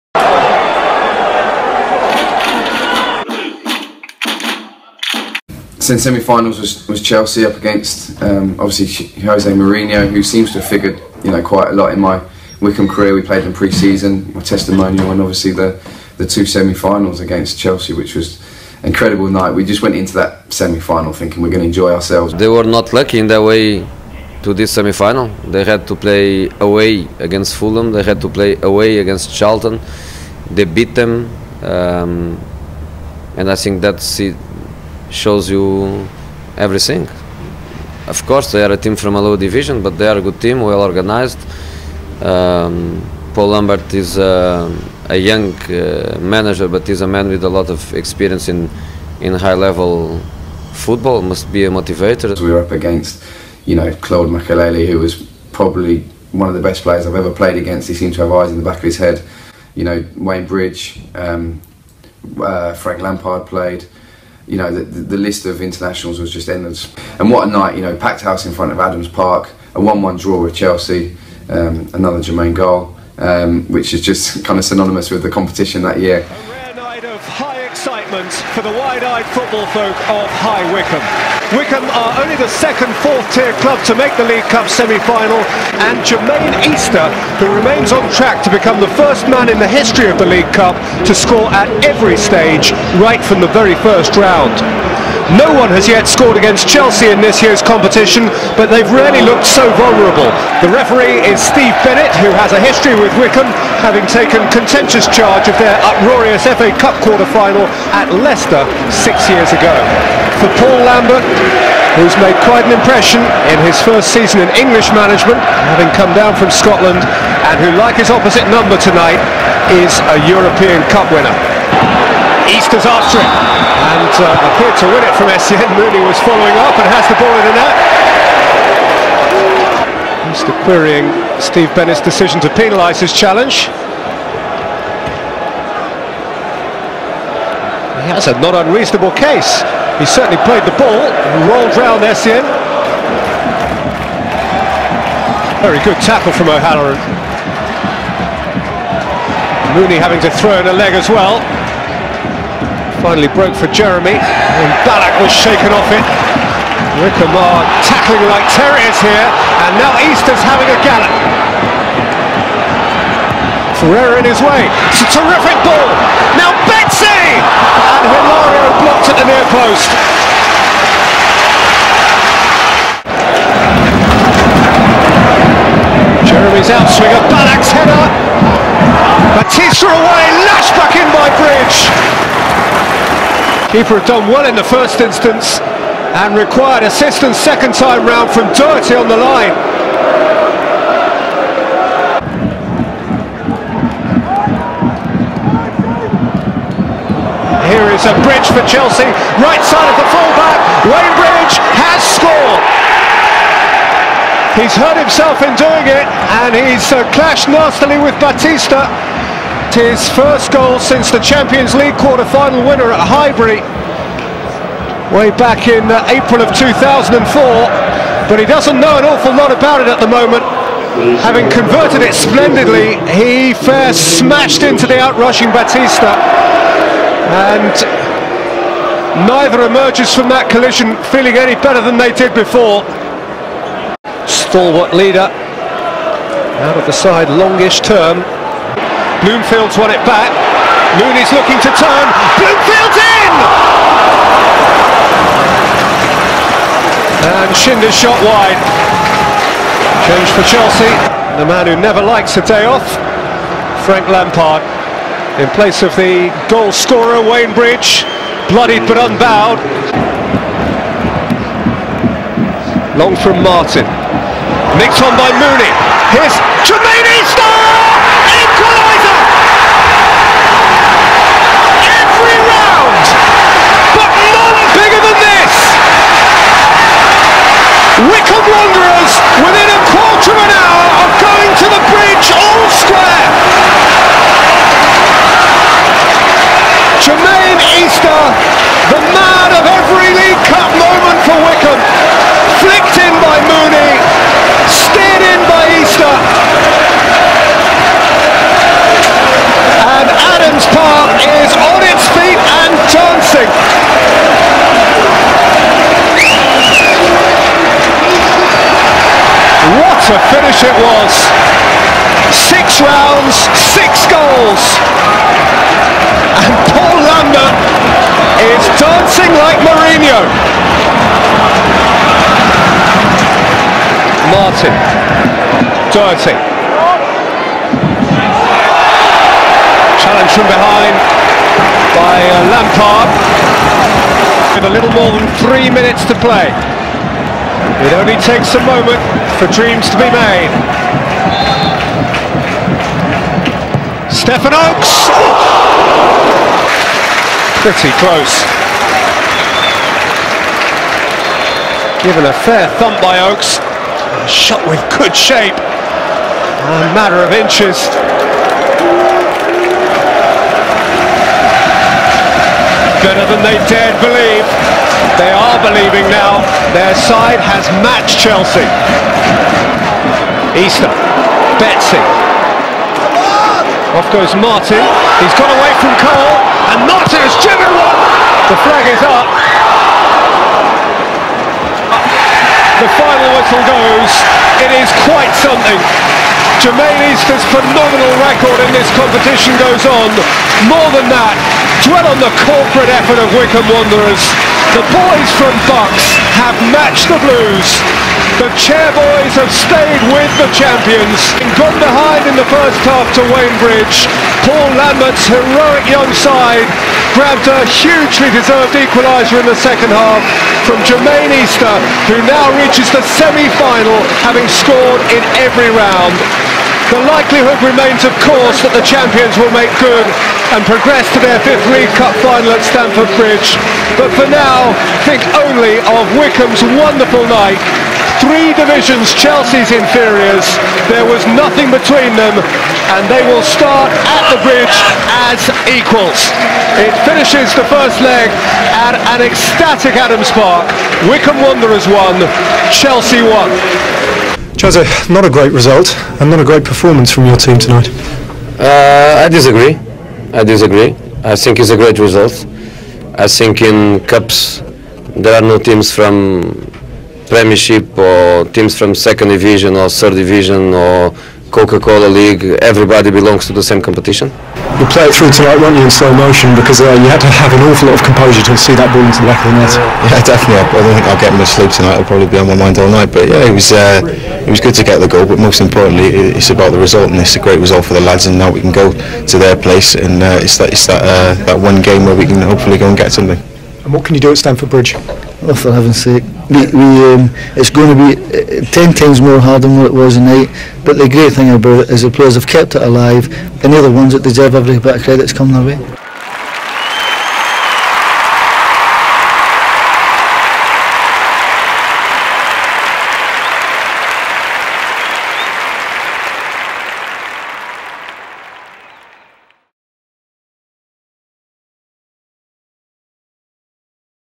10 Years on: Blooms recalls the League Cup run (Part 2)
Matt Bloomfield talks through the first leg of the League Cup Semi-Final in 2007.

Shown at the Ex-Players Association Dinner on 24th November 2017.